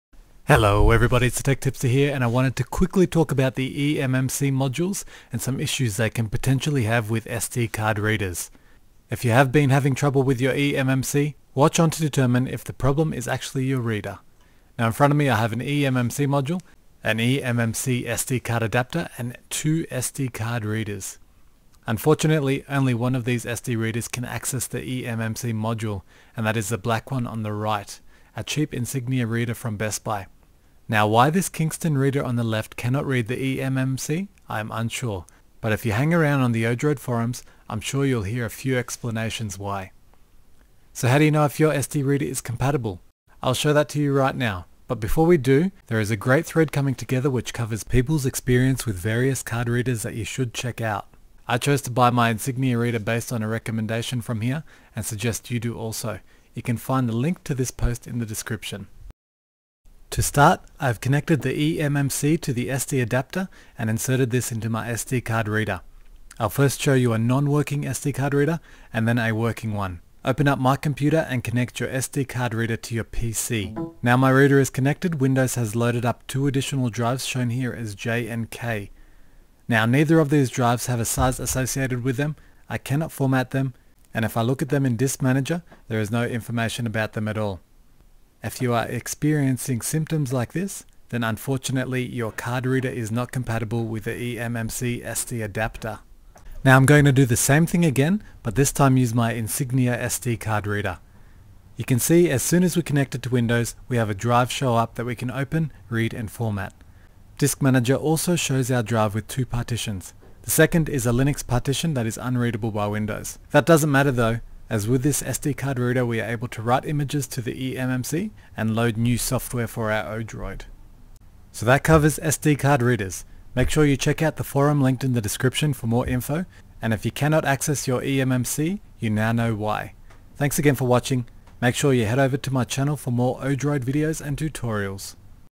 ODroid eMMC SD Card Reader - Is your SD Card Reader Compatible with the eMMC?
ODroid eMMC SD Card Reader - How to determine if your SD Card Reader  is compatible with the eMMC?

If you are using an eMMC with your ODroid, it is possible that you will be unable to read and write from it using a SD Card reader, and the HardKernel supplied SD adapter.

I will show help you determine if your reader is compatible, and if not, where you can find one that is.

Make sure you check out this thread for all the latest updates on compatible SD Card readers

Want to get your own ODroid or eMMC card? Make sure you check out ameriDroid, the official North American supplier for all ODroid related products, visit them here: